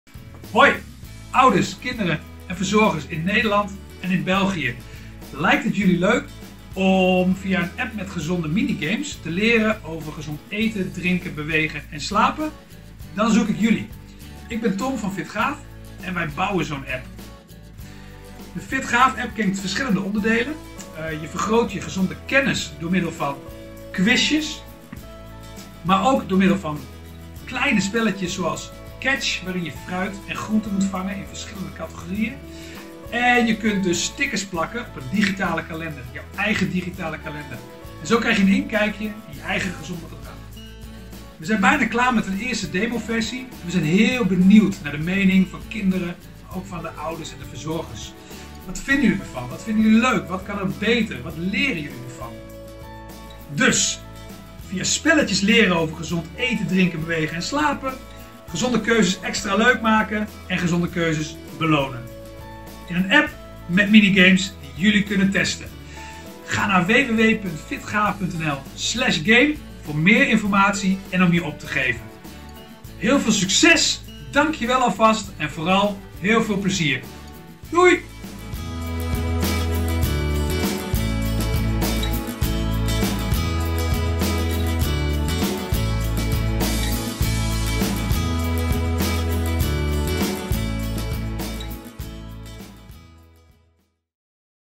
Gratis testen FitGaaf!-app met gezonde minigames
Gezond, met speels gemak? Vanuit FitGaaf! werken we aan een mobiele app met gezonde minigames voor kinderen. Om spelenderwijs te leren over gezond eten, drinken, bewegen en slapen. Een testversie van de app is bijna af! Nu vragen we kinderen en ouders/verzorgers om deze gratis te testen!
Ga naar voor meer info en om de testversie te downloaden!

Te testen voordelen:
- Een leuke en verantwoorde app: spelen en leren.
- Minder strijd rond gezond eten, drinken, bewegen en slapen. Doordat gezonde keuzes worden beloond met digitale stickers en kinderen daardoor zelf voor de gezonde keuze gaan. Zoals de groente opeten.
- Gezonde kennis over bijvoorbeeld fruit (waar komt het vandaan, welke soorten zijn er?)
- De app zet aan tot gezond gedrag, zoals buitenspelen.

Praktische informatie:
- Een testtraject is volledig gratis en duurt maximaal 21 dagen
- Meerdere kinderen in het gezin kunnen meedoen
- Als ouder/verzorger maakt u een hoofdaccount aan. Daarna kunt u meerdere gebruikersaccounts aanmaken.
- U kunt de app ook op een ander toestel (eigen smartphone of bijvoorbeeld tablet) downloaden en uw kinderen daar de FitGaaf!-app laten spelen onder de eigen account.
- Testresultaten worden volledig geanonimiseerd geanalyseerd. Zie voor de volledige privacy-verklaring